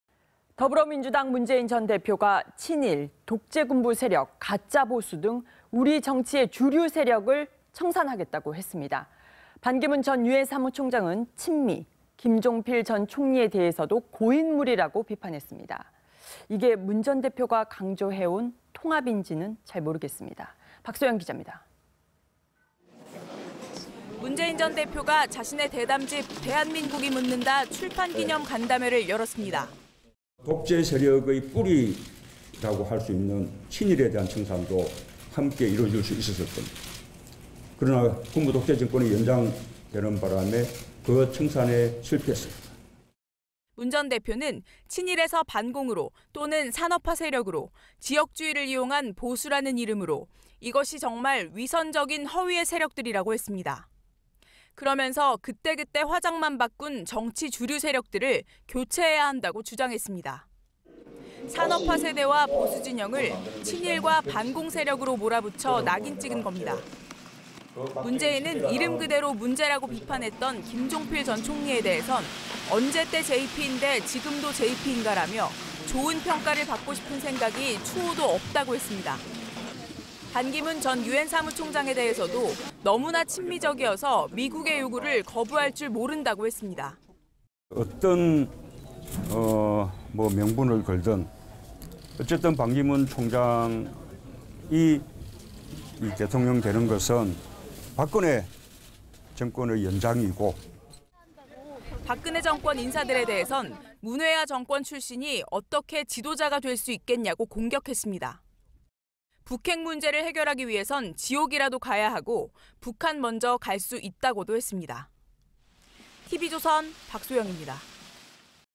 문재인 "친일·독재 등 정치 주류세력 청산"…반기문엔 "친미"
더불어민주당 문재인 전 대표가 친일·독재군부 세력, 가짜 보수 등 우리 정치의 주류세력을 청산하겠다고 했습니다. 반기문 전 유엔 사무총장은 "친미", 김종필 전 총리에 대해서도 "고인 물"이라고 비판했습니다. 이게 문 전 대표가 강조해 온 통합인지 잘 모르겠습니다.